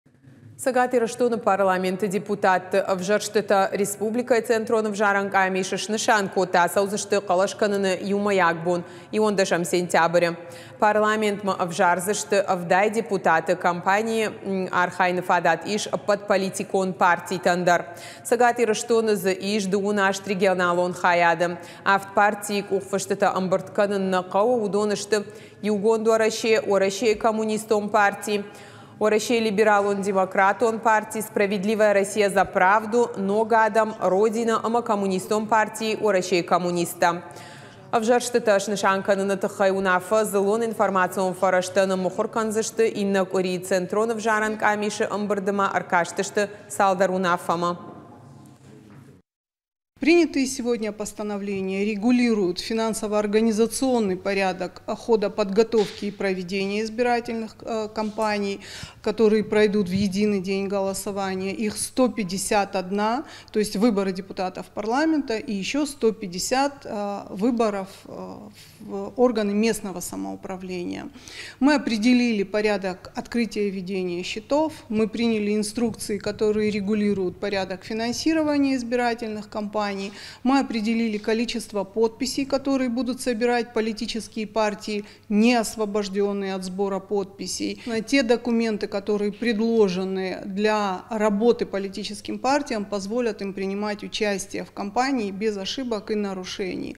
РЦИ Аланийы центрон æвзарæн къамис снысан кодта парламентмæ депутатты æвзæрстытæ
Цæгат Ирыстоны парламенты депутатты æвзæрстытæ республикæйы центрон æвзарæн къамис снысан кодта. Ацæудзысты хъæлæс кæныны иумæйаг бон - 11 сентябры. Парламентмæ æвзардзысты – 70 депутаты. Кампанийы архайыны фадат ис æппæт политикон партитæн дæр. Цæгат Ирыстоны дзы ис 28 регионалон хайады. 7 партийы къухфысттæ æмбырд кæнын нæ хъæуы. Уыдон сты «Иугонд Уæрæсе», Уæрæсейы коммунистон парти, Уæрæсейы Либералон демократон парти, «Справедливая Россия - за правду», «Ног адæм», «Родина» æмæ Коммунистон парти Уæрæсейы коммунисттæ.

Æвзæрстытæ снысан кæныны тыххæй уынаффæ дзыллон информацион фæрæзтæ ныммыхуыр кæндзысты иннæ къуыри. Центрон æвзарæн къамисы æмбырды ма æркастысты цалдæр уынаффæмæ.